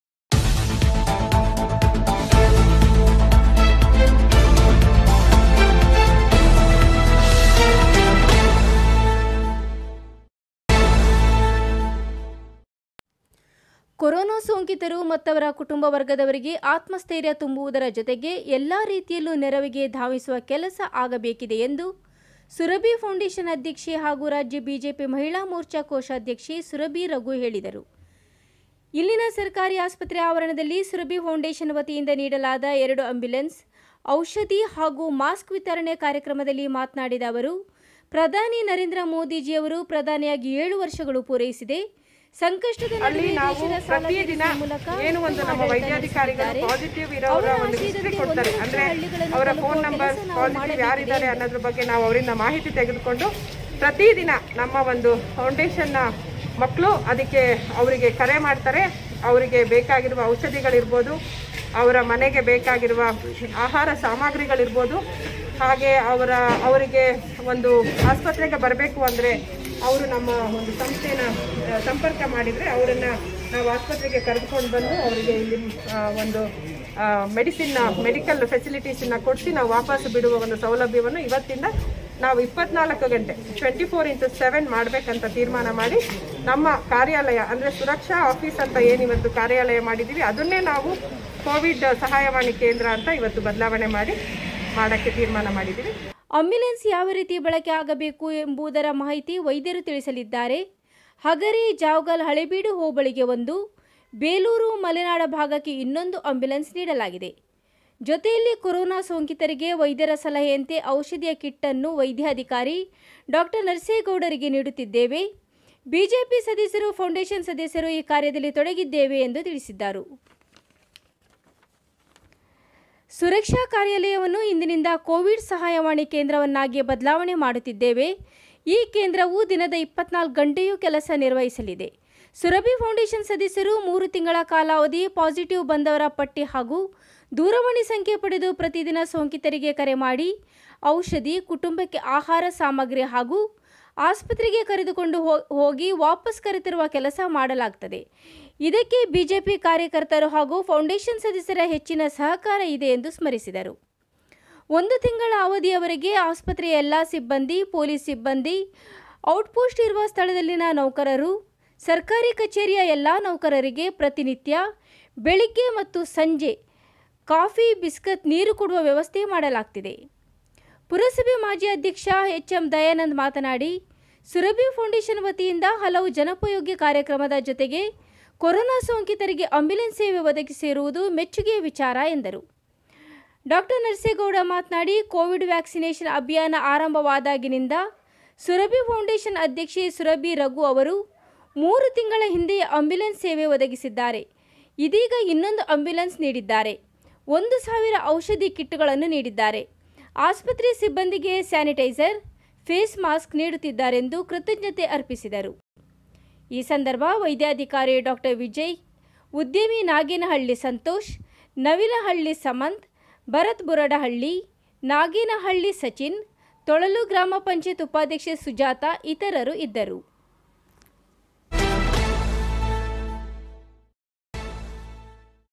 ಬೇಲೂರು ಆಸ್ಪತ್ರೆಗೆ 2 ಆಂಬ್ಯುಲೆನ್ಸ್-ಔಷಧಿ ಕಿಟ್ ಕೊಡುಗೆ ನೀಡಿದ ಸುರಭಿಪೌಂಡೇಶನ್
ಸುರಭಿಪೌಂಡೇಶನ್‍ನಿಂದ 24x7 ಸಹಾಯವಾಣಿ ಕೇಂದ್ರ

ಬೇಲೂರು ಆಸ್ಪತ್ರೆಗೆ 2 ಆಂಬ್ಯುಲೆನ್ಸ್-ಔಷಧಿ ಕಿಟ್ ಕೊಡುಗೆ ಸೋಂಕಿತ ಕುಟುಂಬಕ್ಕೆ ಪಡಿತರ: ನೌಕರರಿಗೆ ಪ್ರತಿನಿತ್ಯ ಕಾಫಿ-ಬಿಸ್ಕತ್.